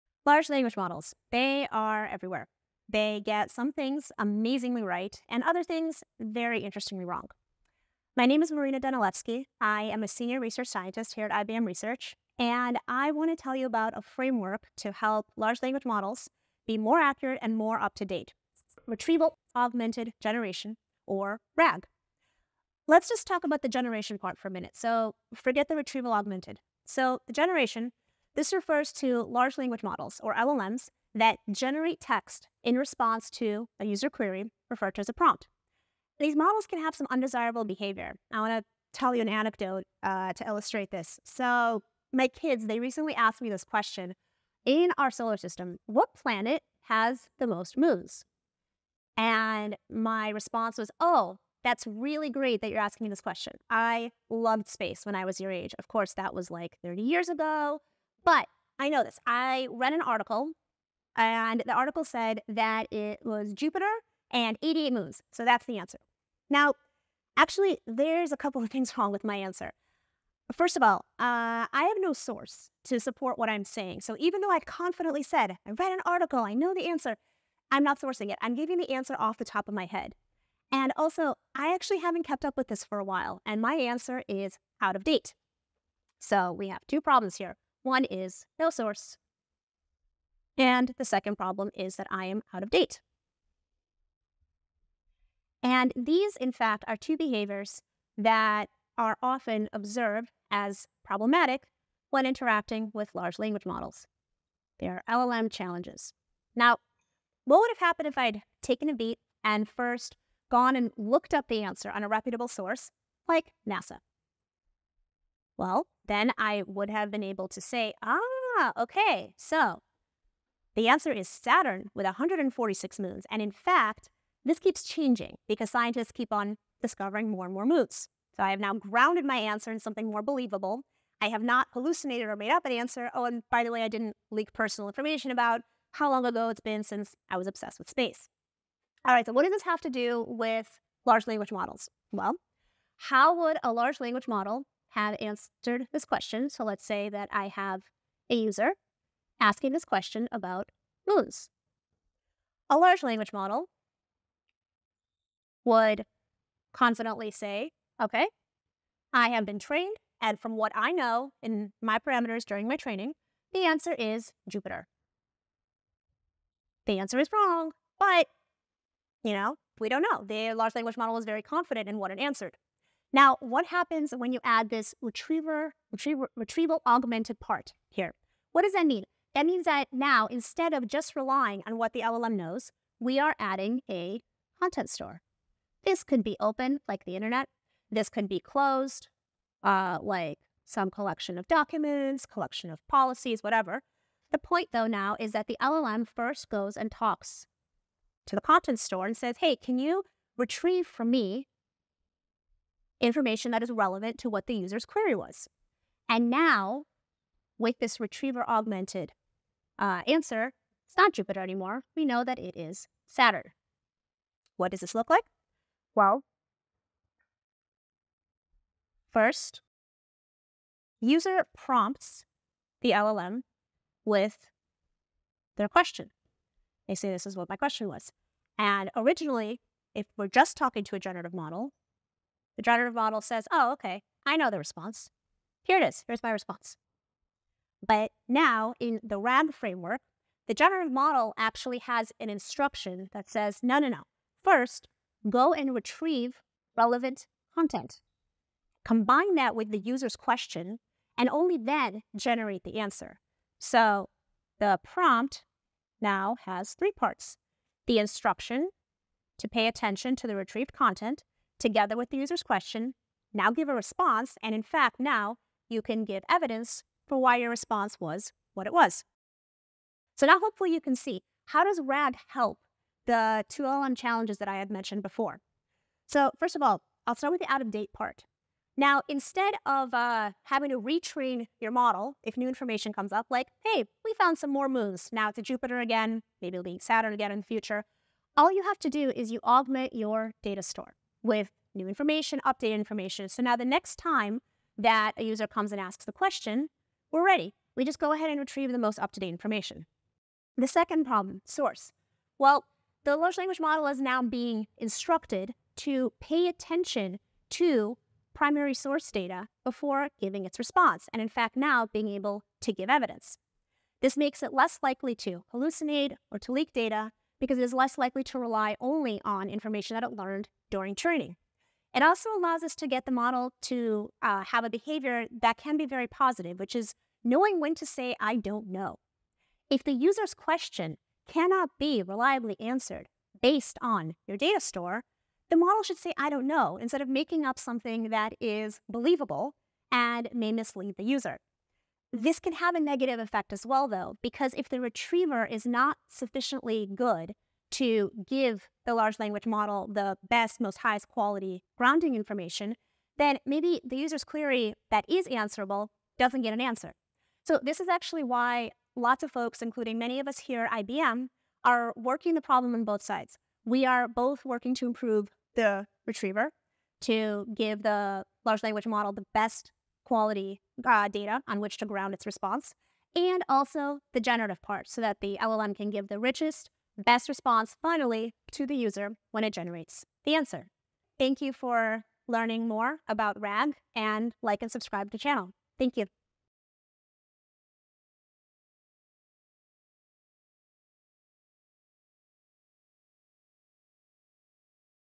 What is Retrieval-Augmented Generation (RAG)?
Large language models usually give great answers, but because they're limited to the training data used to create the model. Over time they can become incomplete--or worse, generate answers that are just plain wrong. One way of improving the LLM results is called "retrieval-augmented generation" or RAG. In this video, IBM Senior Research Scientist Marina Danilevsky explains the LLM/RAG framework and how this combination delivers two big advantages, namely: the model gets the most up-to-date and trustworthy facts, and you can see where the model got its info, lending more credibility to what it generates.

Get weekly AI, cloud, security and sustainability industry news, events and insights.